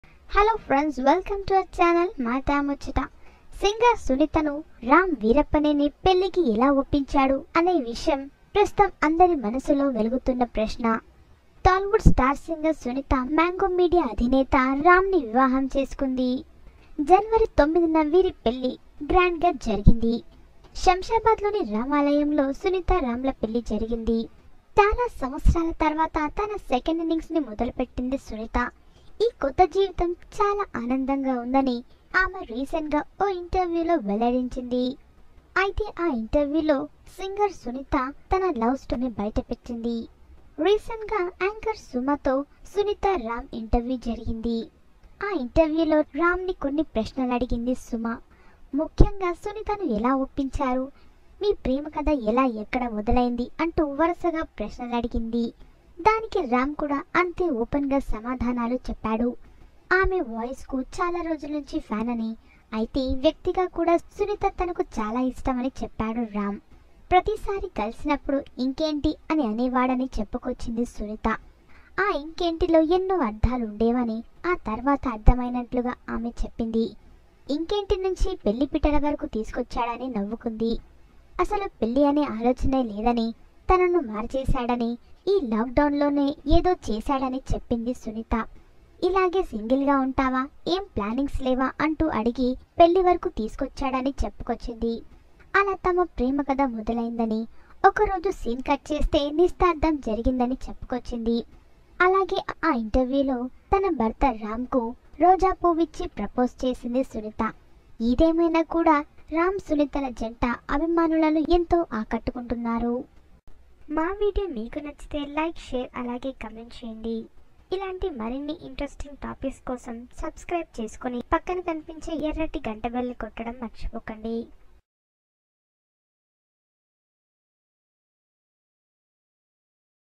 తనను రెండో పెళ్లి చేసుకోమని సింగర్ సునీతని రామ్ ఎలా ఒప్పించాడో తెలుసా Singer Sunitha Ram Proposal
తనను రెండో పెళ్లి చేసుకోమని సింగర్ సునీతని రామ్ ఎలా ఒప్పించాడో తెలుసా Did you know Singer Sunitha Ram Proposal Matter